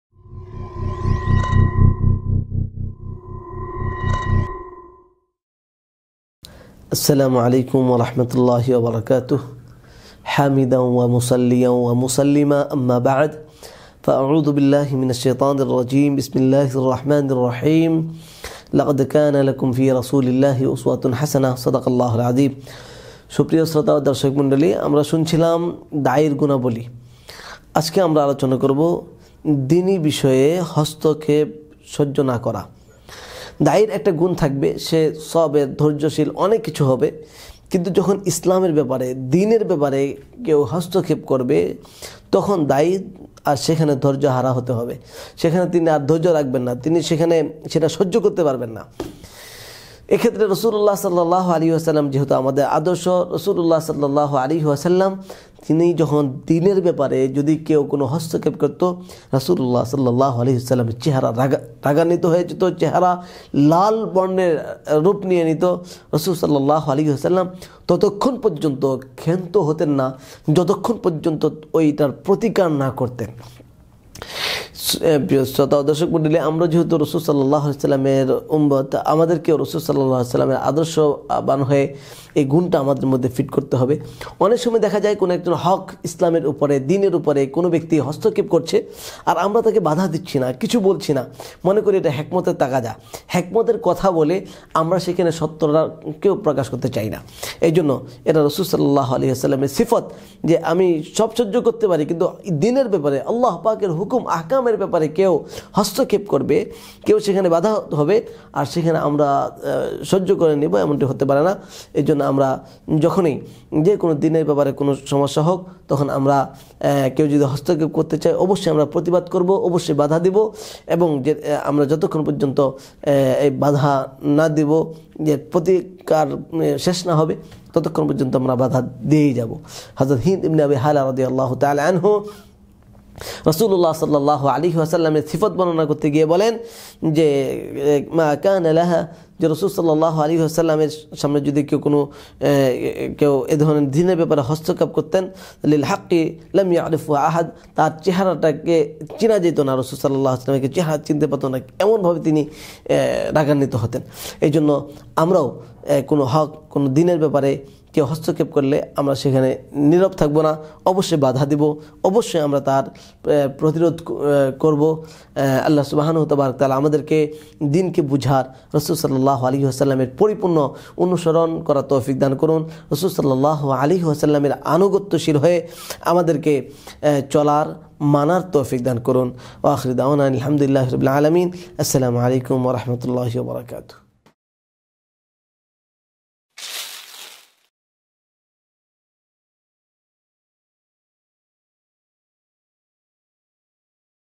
দাঈর গুনাবলী | পর্ব - ১৫ | দ্বীনকে সর্বাপেক্ষা প্রাধান্য দেওয়া | Mufti Jubaer Ahmad
দাঈর গুনাবলী
পর্ব - ১৫
আজকের বিষয়ঃ  দ্বীনকে সর্বাপেক্ষা প্রাধান্য দেওয়া
আলোচকঃ মুফতি যুবায়ের আহমাদ
পরিচালক, ইসলামী দাওয়াহ ইন্সটিটিউট ।

Speaker: Shayekh Mufti Jubaer Ahmad
Principal, Islami Dawoah Institute, Dhaka .

Stay with us to get updated with latest Bengali Islamic contents.